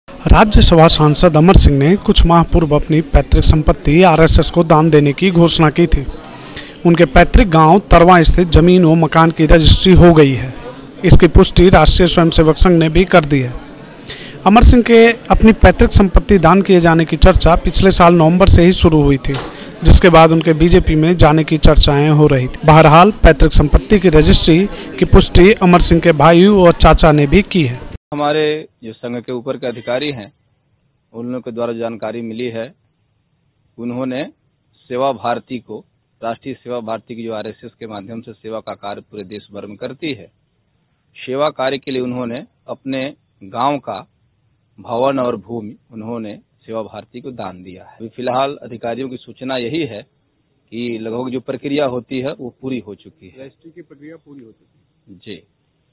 अमर सिंह द्वारा संघ को दान दी गई जमीन की हुई रजिस्ट्री, आरएसएस ने की पुष्टि
आजमगढ़ : राज्यसभा सांसद अमर सिंह  ने कुछ माह पूर्व अपनी पैतृक संपत्ति आरएसएस को दान देने की घोषणा की थी. उनके पैतृक गांव तरवां स्थित जमीन व मकान की रजिस्ट्री हो गई है.  इसकी पुष्टि राष्ट्रीय स्वयं सेवक संघ ने भी कर भी दी है.